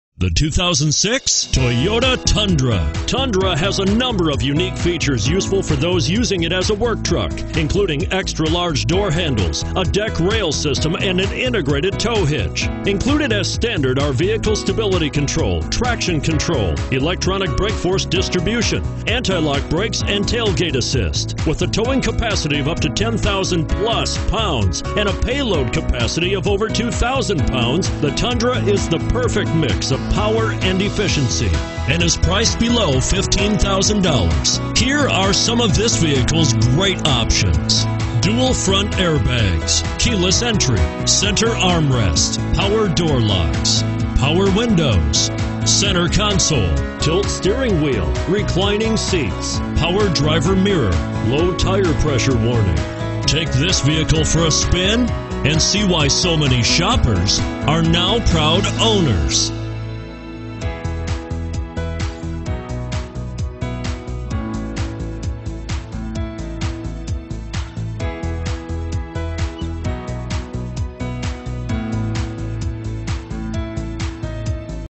Toyota Tundra Wolfforth TX P04227
2006 Toyota Tundra ACCESSCAB V6 AUTO SR5

This one has the 4.0L V6 engine and is a 4x2. Toyota realized that consumers expect certain things when they buy a Toyota truck: excellent build quality, class-leading reliability and well-designed interiors. Inside, you'll be spoiled with cruise control, carpeted floors, a digital quartz clock, dual 12-volt auxiliary power outlets, remote keyless entry, a tilt steering wheel, and more! This Tundra is the perfect weekend project truck for those trips to the home improvement store! You're safety wasn't overlooked, either! Check out the extensive list of safety features that include 3-point seat belts for all seat positions, 4-wheel anti-lock brakes with brake assist, driver and front passenger dual-stage airbags with passenger airbag cutoff switch, an energy-absorbing steering column, side-impact door beams, and the list goes on! Toyota's first full-size truck is a very good one, especially for the general consumer rather than the contractor or construction worker. Our Tundra should be in your driveway!